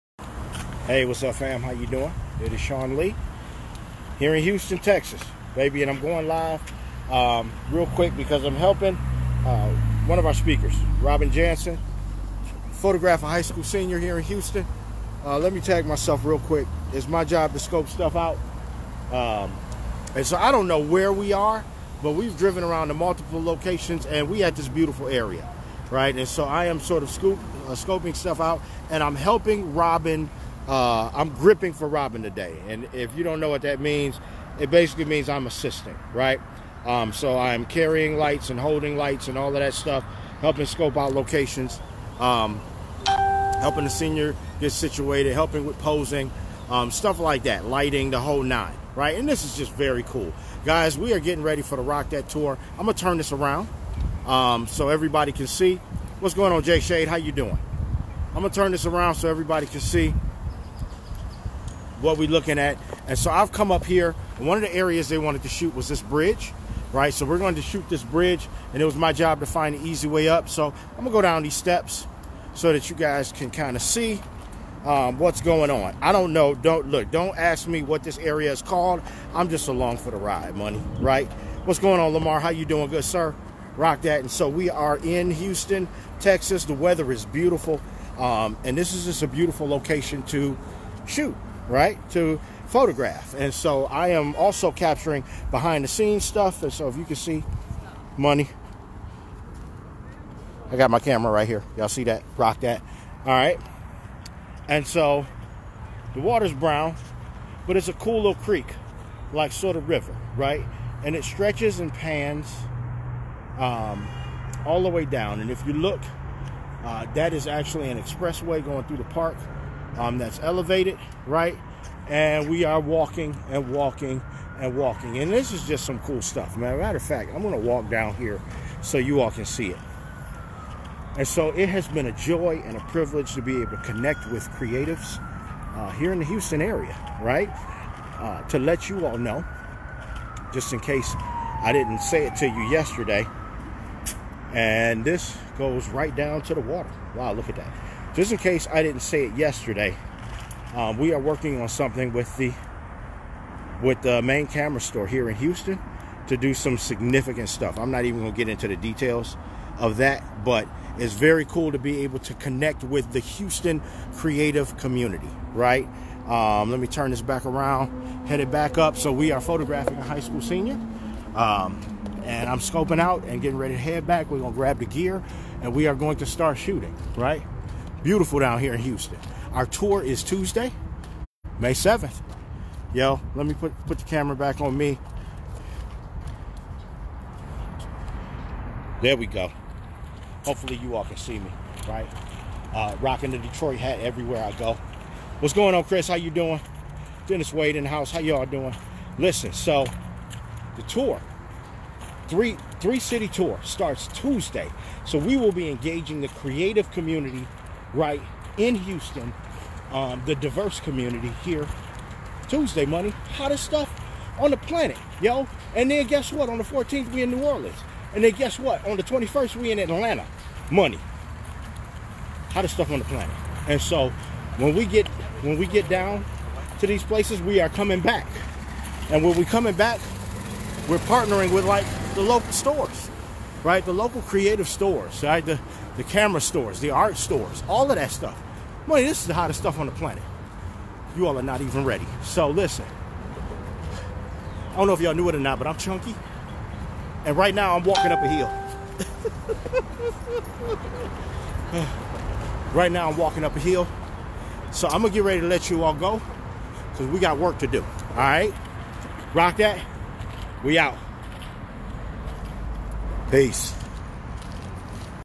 Hanging with Professsional Photographer Robin Janson from Houston, Tx. -- (We Are MAPP)
In Houston w/ Robin Janson one of our ROCK THAT Tour speakers photographing a High School Senior! Very Cool Stuff! rockthattour.com